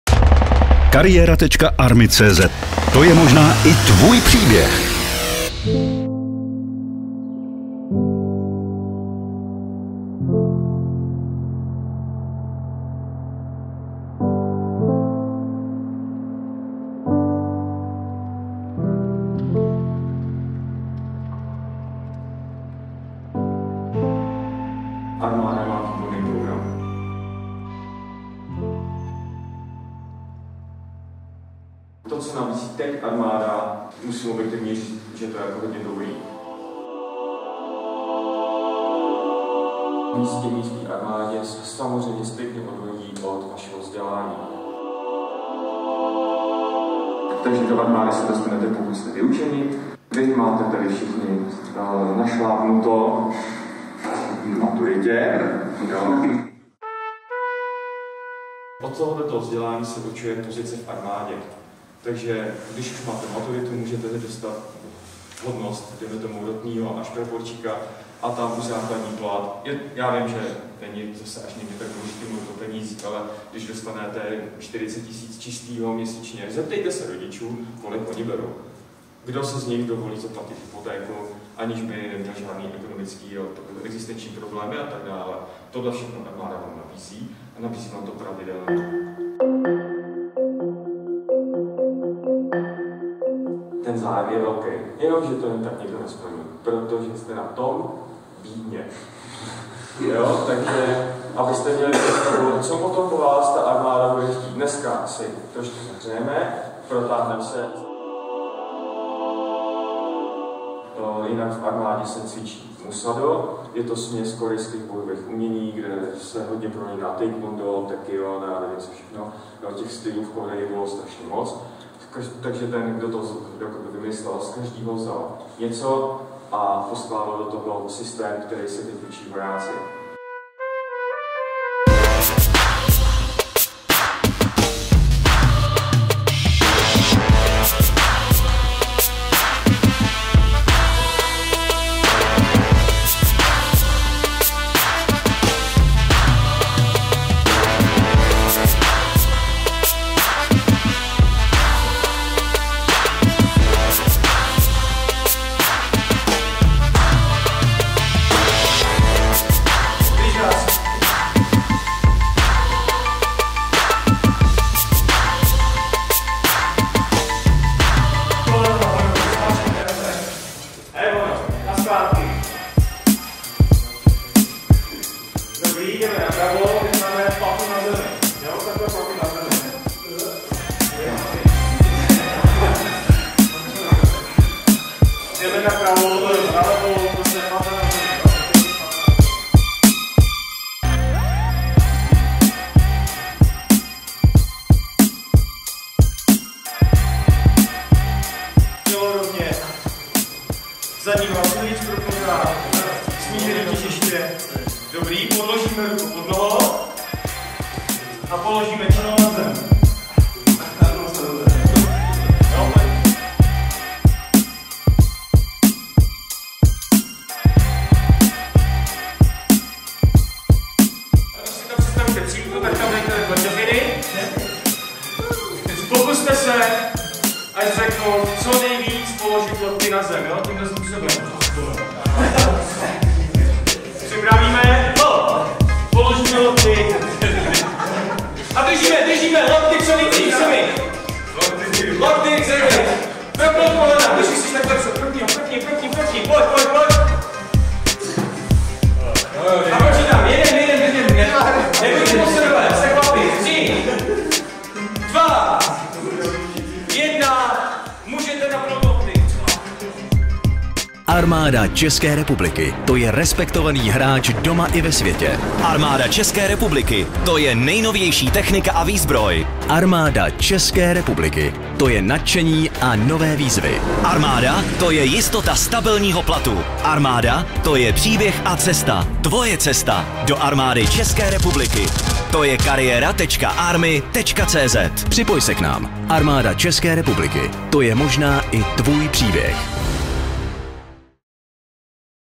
Bojová umění v AČR – lekce s Františkem Jeřábkem (1. díl)
/// Nadrotmistr Armády ČR František Jeřábek je specialistou na bojová umění. Jeho aktuálním úspěchem jsou dvě stříbrné medaile z mistrovství světa v taekwondu.

Teď vám František Jeřábek naordinuje „self-defense" kurz a poví i něco o službě v AČR. Začínáme rychlou rozcvičkou. Připravte se, 호신술 je tady!

Nadrotmistra Jeřábka můžete osobně potkat na Rekrutačním pracovišti České Budějovice, protože je jedním z armádních rekrutérů! Rekrutační místa přehledně najdete na kariera.army.cz.